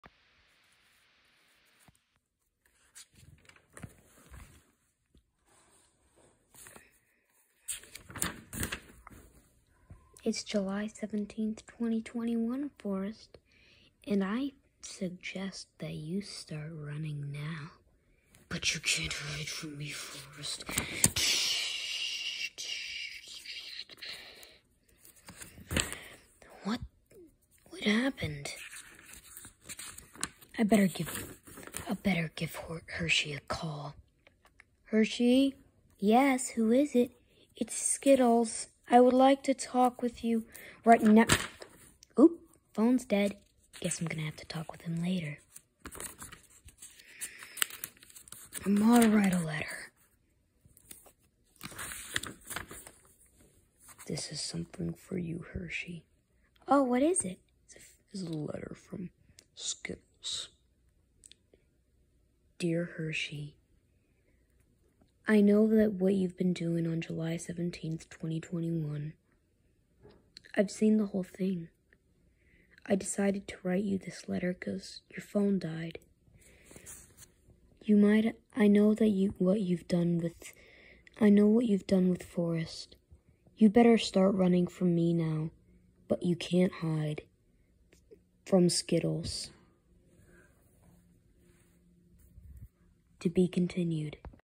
Skittles warning Hershey about what Hershey did
It all started on July 17 2021 when forest was caught he was made into a new flap or of chocolate. Skittles saw the whole thing on YouTube so he called Hershey but his phone died so Skittles wrote Hershey a letter warning Hershey about what will happen on August 15 2021.